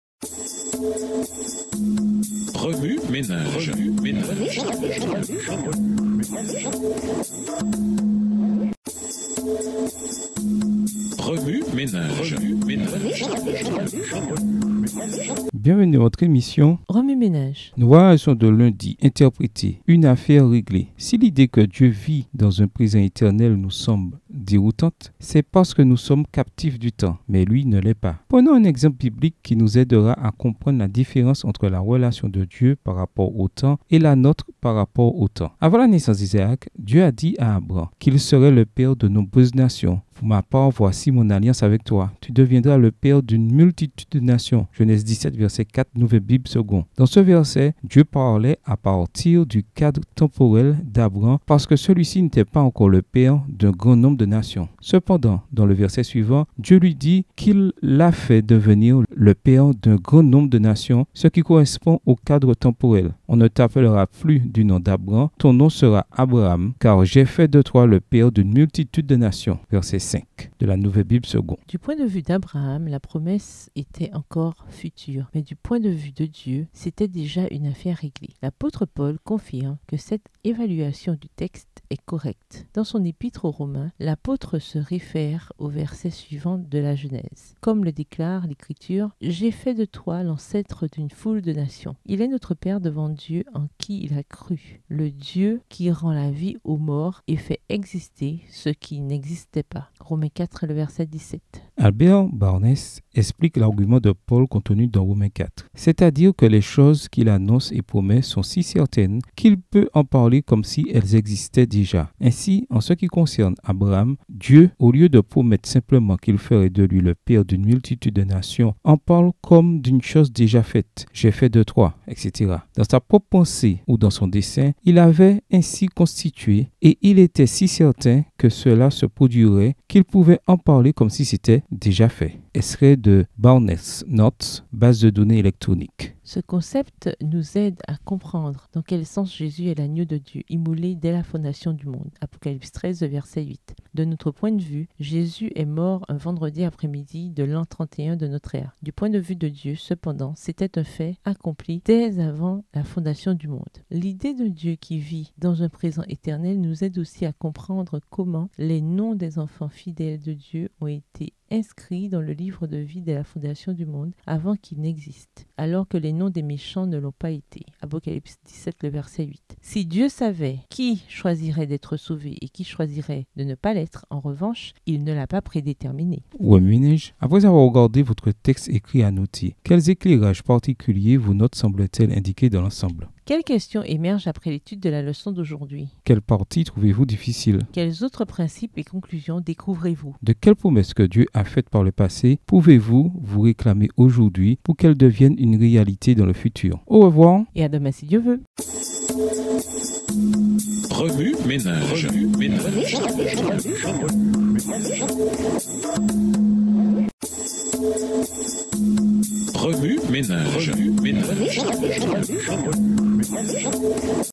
Leçon 11 : Questionnaire Des Jeunes Adultes, Lundi 5 Décembre 2022, Une affaire réglée
interpréter
Une affaire conclue
L'idée de Dieu vivant dans un éternel présent nous paraît déroutante car nous sommes captifs du temps et il ne l'est pas. Prenons un exemple biblique qui nous aidera à comprendre la différence entre la relation de Dieu au temps et la nôtre. Avant la naissance d'Isaac, Dieu a dit à Abram qu'il serait le père de plusieurs nations : « Quant à moi, voici, mon alliance est avec toi, et tu seras le père de plusieurs nations » ( Gen. 17:4, NKJV , emphase fournie). Dans ce verset, Dieu parlait de l'époque d'Abram parce qu'Abraham n'était pas encore le père de nombreuses nations. Dans le verset suivant, cependant, Dieu lui a dit qu'il avait faitlui le père de beaucoup de nations, qui était du temps de Dieu : « Tu ne t'appelleras plus Abram, mais ton nom sera Abraham ; car je t'ai établi père d'une multitude de nations » (v. 5, NKJV, emphase fournie).

Du point de vue d'Abraham, la promesse était encore future, mais du point de vue de Dieu, c'était déjà chose faite ! L'apôtre Paul confirme que cette évaluation du texte est correcte. Dans le livre de Romains, l'apôtre fait référence à ces versets de la Genèse : « (Comme il est écrit : 'Je t'ai établi père de plusieurs nations'). Il est notre père en la présence de Dieu en qui il a cru - le Dieu qui fait revivre les morts et appelle les choses qui n'existent pas encore comme si elles existaient déjà » ( Rom. 4:17, NET Bible, emphase fournie).

Albert Barnes a expliqué l'argument de Paul dans Romains 4 : « C'est-à-dire que les choses qu'il prédit et promet sont si certaines qu'il peut en parler comme déjà existantes. Ainsi, par rapport à Abraham, Dieu, au lieu de simplement promettre qu'il ferait de lui le père de nombreuses nations, en parle comme il l'a déjà fait, "Je t'ai créé", etc. Dans son propre esprit, ou dessein, il avait ainsi le constituait-il, et il était si certain que cela arriverait, qu'il pouvait en parler comme déjà fait. (Notes de Barnes, Base de données électronique, Copyright © 1997, 2003, 2005, 2006 par Biblesoft, Inc.)

Ce concept nous aide à comprendre dans quel sens Jésus est l'Agneau de Dieu immolé depuis la fondation du monde ( Apoc. 13:8 ). De notre point de vue, Jésus est mort un vendredi après-midi de l'an 31. Du point de vue de Dieu, cependant, c'était un fait accompli avant la fondation du monde. L'idée d'un Dieu qui vit dans un présent éternel nous aide aussi à comprendre comment les noms des enfants fidèles de Dieu étaient écrits dans le livre de vie depuis la fondation du monde, avant qu'ils n'existent, alors que les noms des méchants ne l'étaient pas ( Apoc . .17:8 ). Dieu savait qui choisirait d'être sauvé et qui ne le ferait pas, mais Il ne l'a pas prédéterminé.